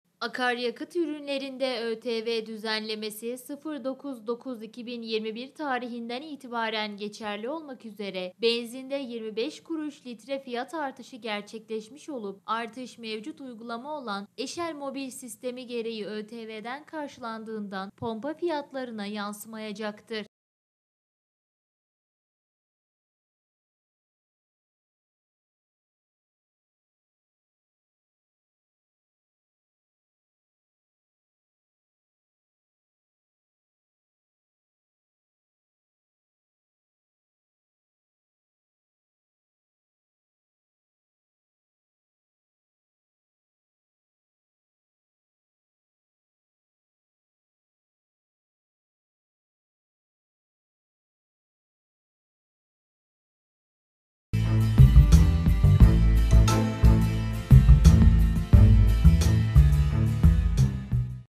NEVŞEHİR - BİR ZAM DAHA GELDİ
Son dakika. Benzinin litre fiyatına yarından itibaren geçerli olmak üzere 25 kuruş zam geldi.